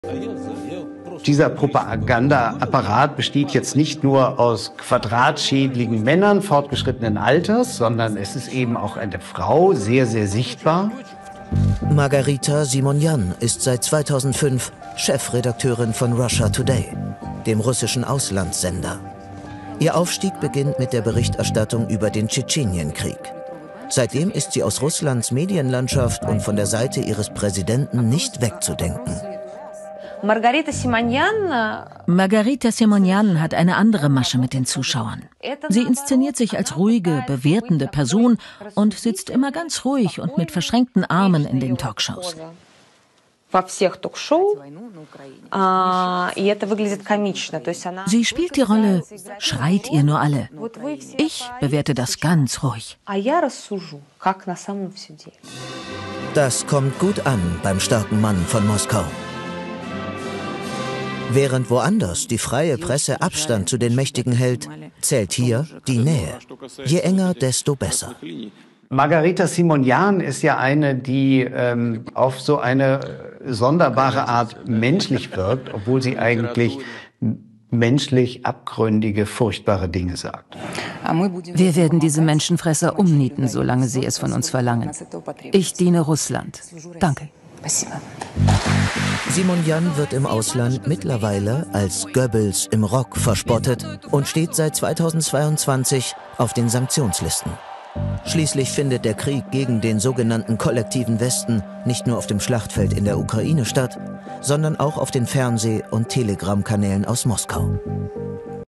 снял фильм о Маргарите_Симоньян.
Ее там назвали "Геббельском в юбке".

«Пропагандистский аппарат Путина состоит теперь не только из квадратноголовых мужчин преклонного возраста. Там очень-очень заметна одна женщина».

С таких слов начинается документальный фильм о Маргарите Симоньян.